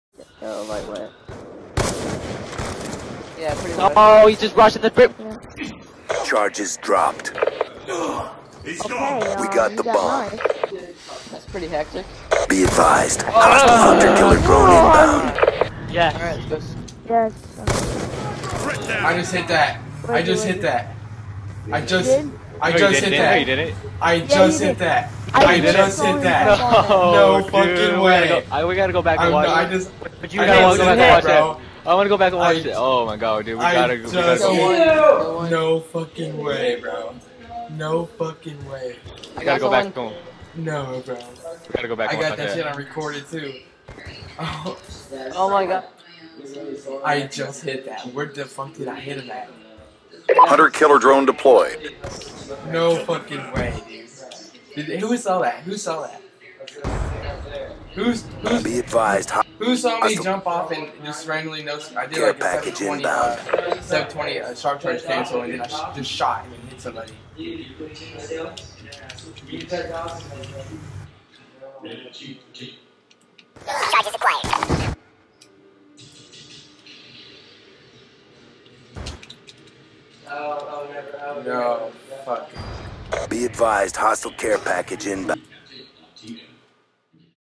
Best Two Piece Trickshot Ever!!!
Whats up guys playing some search and destroy and i hit this awesome clip so bad ass.